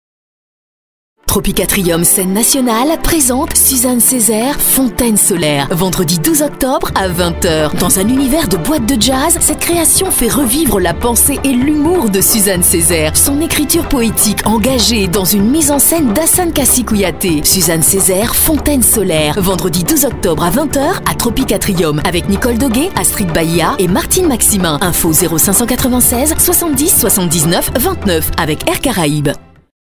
Bande-annonce Suzanne Césaire, fontaine solaire
La pièce adaptée pour le théâtre par Daniel Maximin, "Suzanne Césaire, fontaine solaire", est de retour en Martinique ! Découvrez là à Tropiques Atrium, le vendredi 12 octobre, à 20h !

Portés à la scène pour la première fois, ces écrits de l’intellectuelle et écrivaine martiniquaise Suzanne Césaire jouèrent un rôle majeur dans l’émergence des Antilles contemporaines..Cette écriture poétique, engagée, presque révolutionnaire, révélée par la mise en scène d’Hassane Kassi Kouyaté, trouve ici une résonnance d’une étrange actualité et d’une grande universalité.